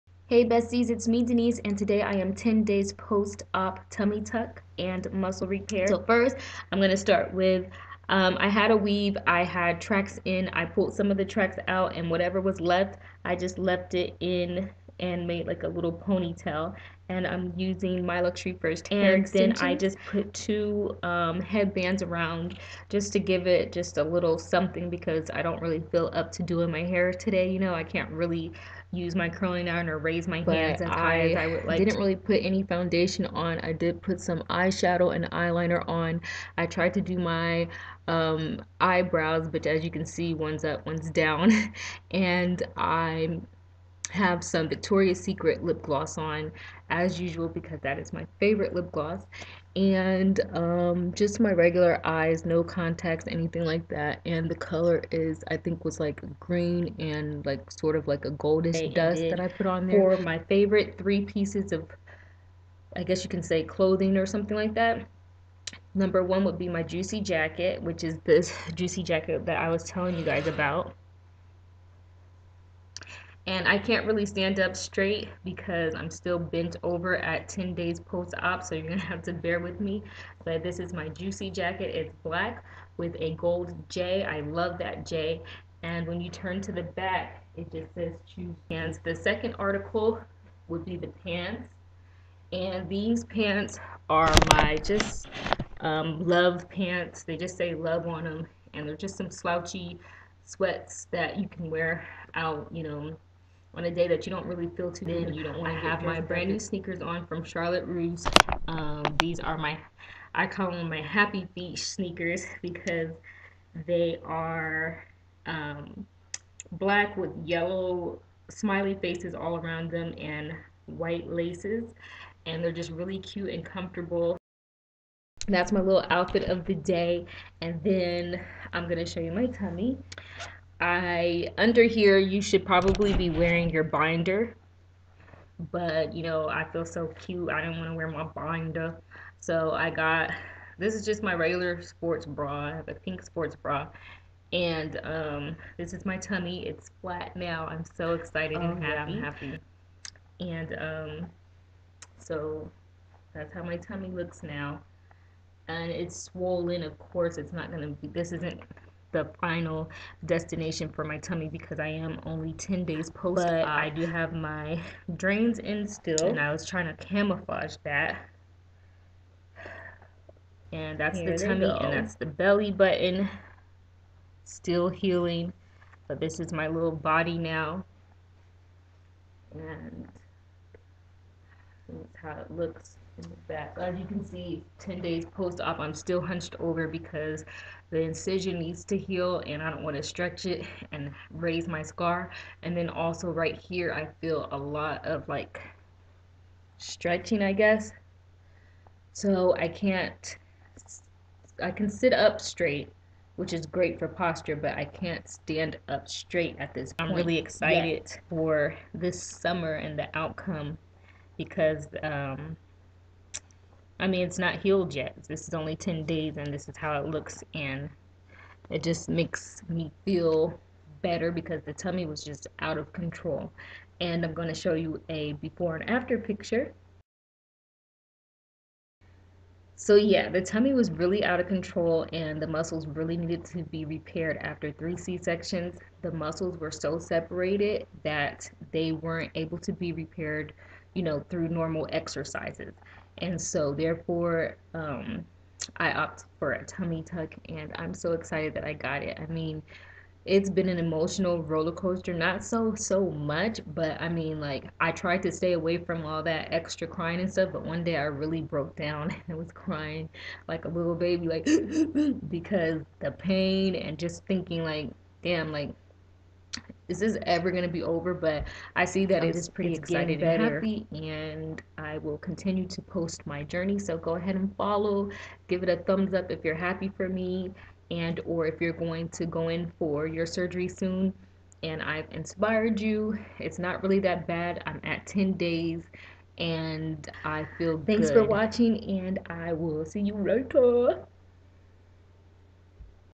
Transformation Tuesday OOTD 10-11 Days Post OP Tummy Tuck, Reveal, Drains MRSD3NISE
I keep saying 10 days, but it's actually 11 days! Post op!  Tummy Tuck, Muscle Repair, Hernia Repair, Liposuction, & Public Contour

What I'm wearing:
hair extensions in Jet Black wefts
2 headbands, just to give some definition to my hairline and ponytail
Eyeshadow from the Naked 2 & Hard Candy pallets [Green n Goldish dust]
Eyeliner, Eyebrows, Victoria Secret Lip Gloss
Choose Juicy [Couture] Jacket
LOVE slouchy, casual, sweat pants
Happy Feet sneakers, black and yellow smiley faced from Charlotte Russe

Tummy shown on 11 days post op, with healing belly button, tummy is still swollen and belly button is healing,  Drains are also still in and hanging as shown.  I can't stand up totally straight but it has gotten better.

Before and After picture also shown.